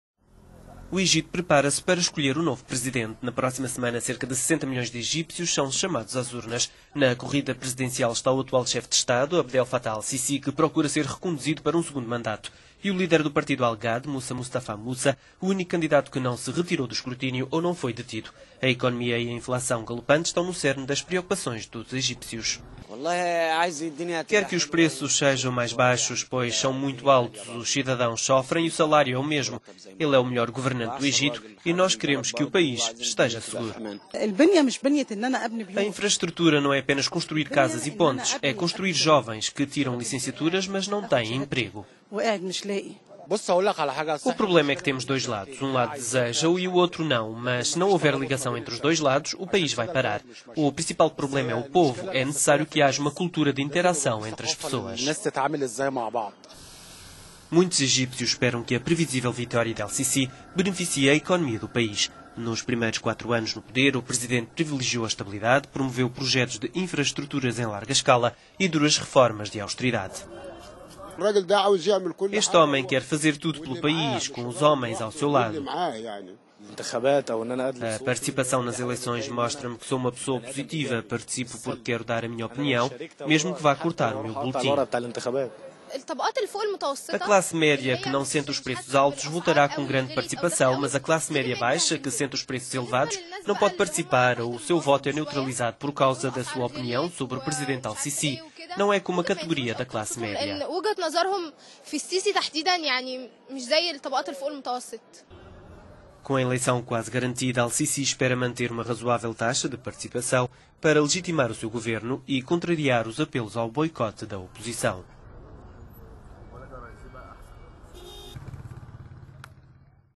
Egito prepara-se para a reeleição de Al-Sisi
Na corrida presidencial está o atual chefe de Estado, Abdel-Fattah Al-Sisi, que procura ser reconduzido para um segundo mandato, e o líder do Partido al-Ghad, Moussa Mostafa Moussa: o único candidato que não se retirou do escrutínio ou não foi detido.…